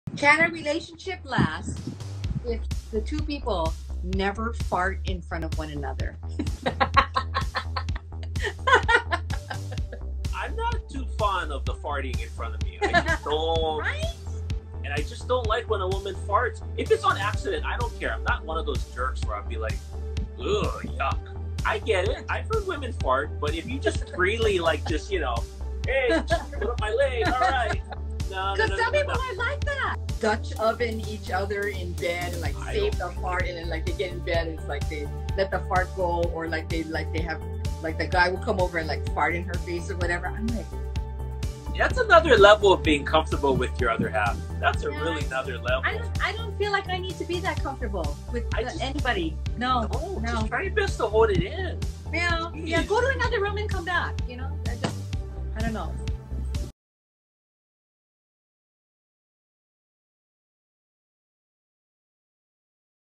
Farting with Your Other Half?
In our recent Instagram live “Valentine’s Special” we had our guest Co-Host Esme Infante join Damon, to answer all questions on love and relationships! And boy did they go deep! Farts and all!